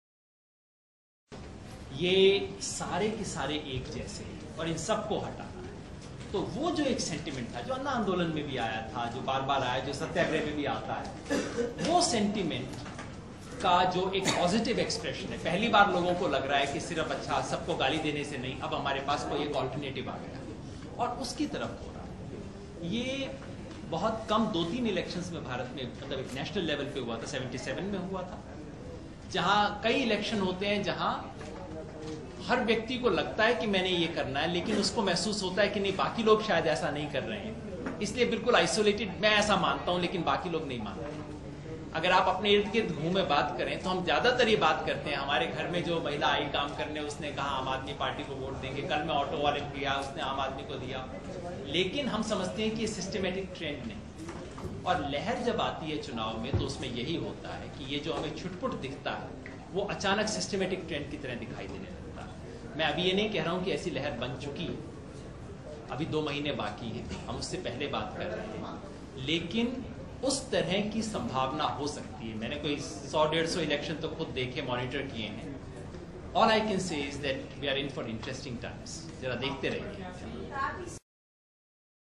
Now people see AAP as a positive alternative: Yadav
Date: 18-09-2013

Aam Aadmi Party leader Yogendra Yadav has said that it is a good thing for AAP as people of Delhi take it as a positive alternative.

He was interacting with media with other party leaders on the recent survey conducted on Delhi Assembly polls in which AAP performance has shown satisfactory.